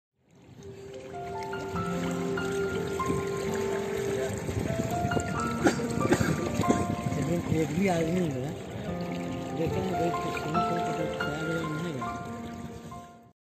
Do what makes you happy!
A good life is when you assume nothing, do more, need less, smile often, dream big, laugh a lot, and realize how blessed you are. ❤

This is the early morning ganges view from Haridwar. A short much travel break was needed after the months of lockdown. The water was a bit cold but it was all worth it. 😁